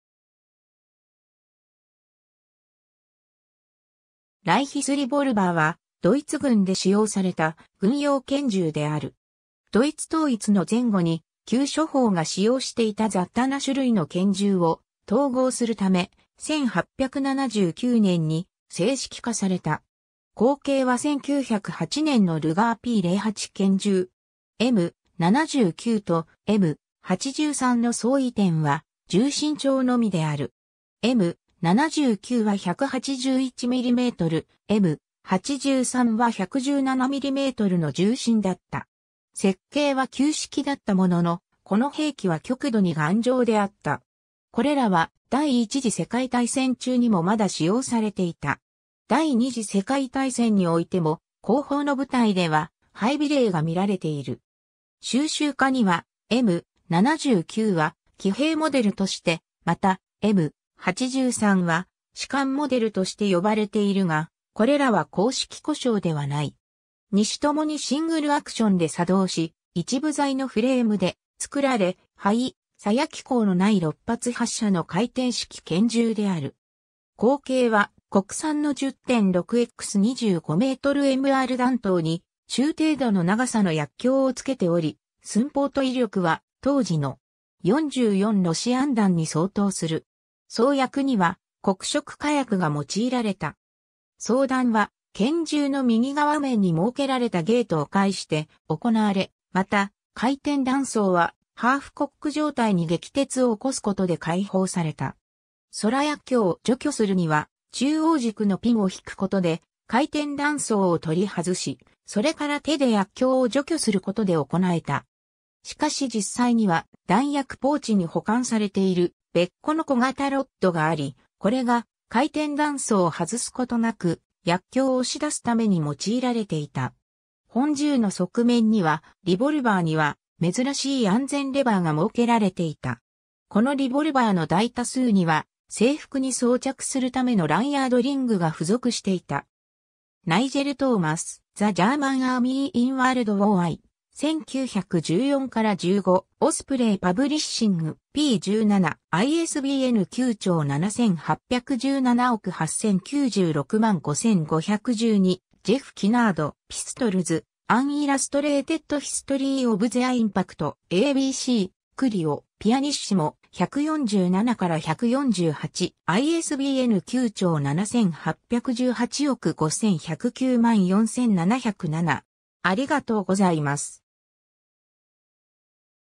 ライヒスリボルバー
ライヒスリボルバー（ドイツ語: Reichsrevolver）は、ドイツ軍で使用された軍用拳銃である。
ドイツ統一の前後に旧諸邦が使用していた雑多な種類の拳銃を統合するため、1879年に制式化された。
後継は1908年のルガーP08拳銃。
 M/79とM/83の相違点は、銃身長のみである。
M/79（Modell 1879）は181mm、M/83（Modell 1883）は117mmの銃身だった。
設計は旧式だったものの、この兵器は極度に頑丈であった。
これらは第一次世界大戦中にもまだ使用されていた。
第二次世界大戦においても、後方の部隊では配備例が見られている。
 収集家にはM/79は「騎兵モデル」として、またM/83は「士官モデル」として呼ばれているが、これらは公式呼称ではない。
 二種ともにシングルアクションで作動し、一部材のフレームで作られ、排莢機構の無い6発発射の回転式拳銃である。
口径は国産の10.6x25mmR弾頭に中程度の長さの薬莢を付けており、寸法と威力は当時の.44ロシアン弾に相当する。
装薬には黒色火薬が用いられた。
 装弾は拳銃の右側面に設けられたゲートを介して行なわれ、また回転弾倉はハーフ・コック状態に撃鉄を起こすことで開放された。
空薬莢を除去するには、中央軸のピンを引くことで回転弾倉を取り外し、それから手で薬莢を除去することで行えた。
しかし実際には、弾薬ポーチに保管されている別個の小型ロッドがあり、これが回転弾倉を外すことなく薬莢を押し出すために用いられていた。
本銃の側面には、リボルバーには珍しい安全レバーが設けられていた。
 このリボルバーの大多数には、制服に装着するためのランヤード・リングが付属していた。
 ^ Nigel Thomas (2012). The German Army in World War I (1): 1914-15. Osprey Publishing. p. 17. ISBN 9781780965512 ^ Jeff Kinard (2003). Pistols: An Illustrated History of Their Impact. ABC-CLIO. pp. 147-148. ISBN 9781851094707